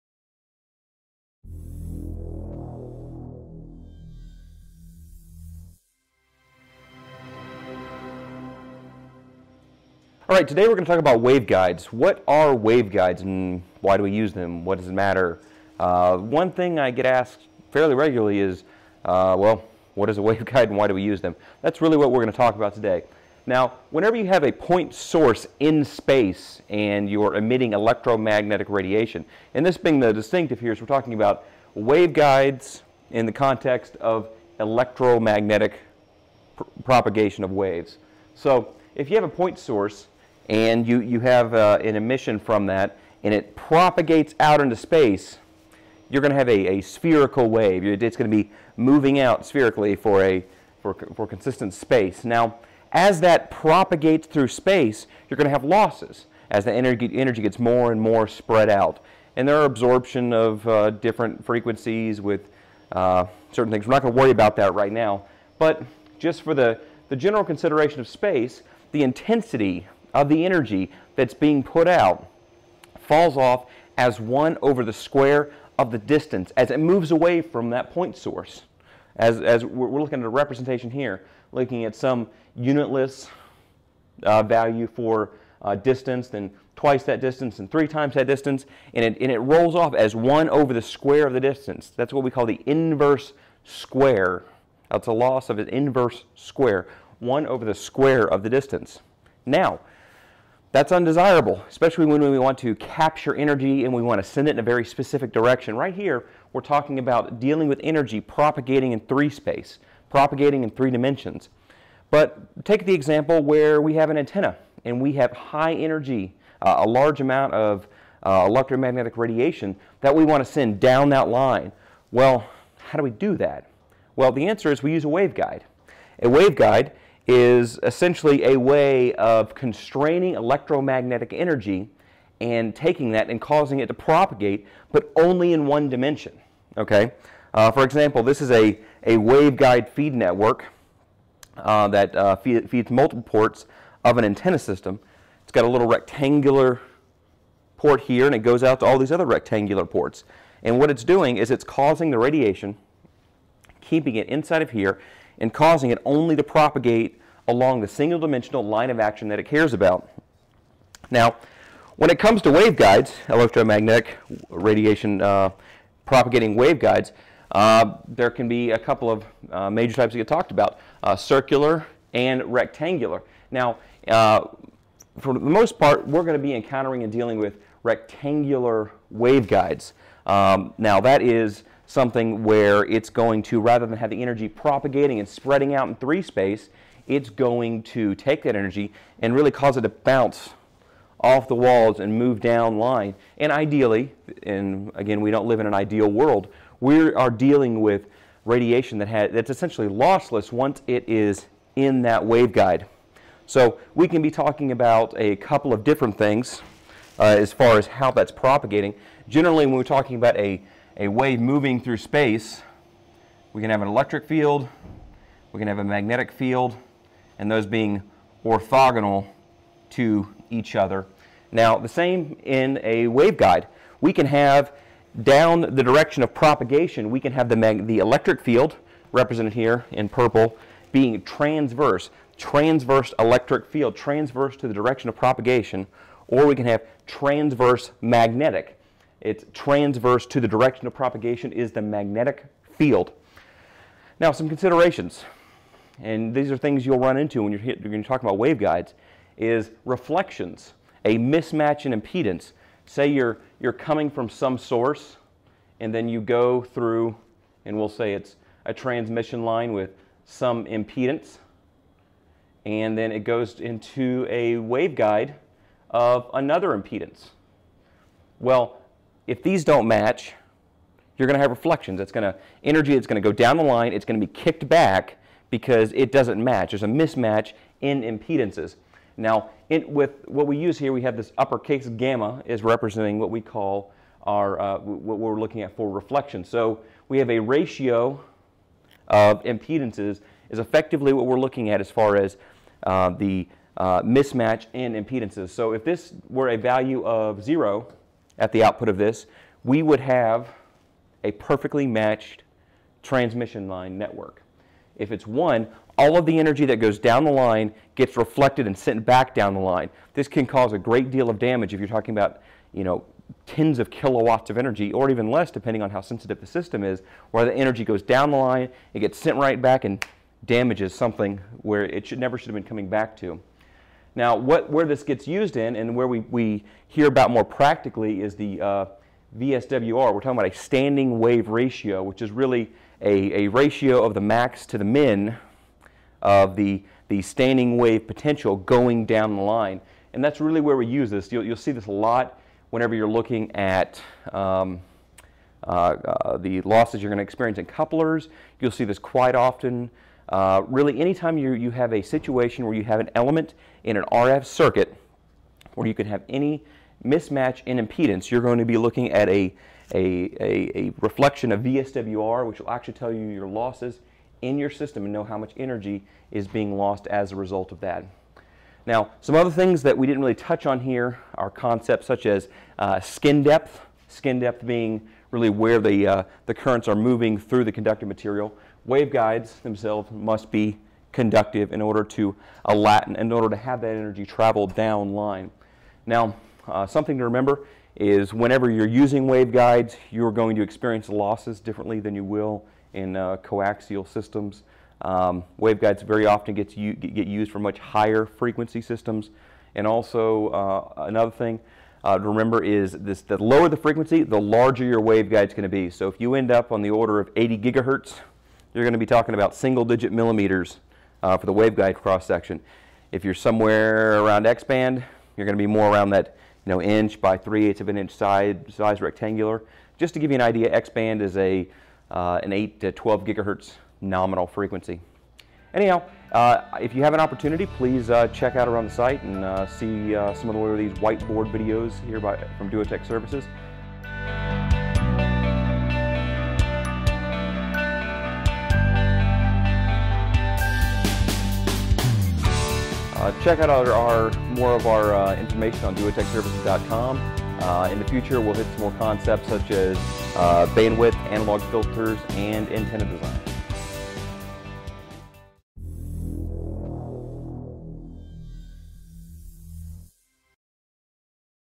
Waveguides - Weekly Whiteboard
This video is about the basic concept of waveguides for electromagnetic wave propagation.  These structures allow for nearly lossless propagation of RF energy.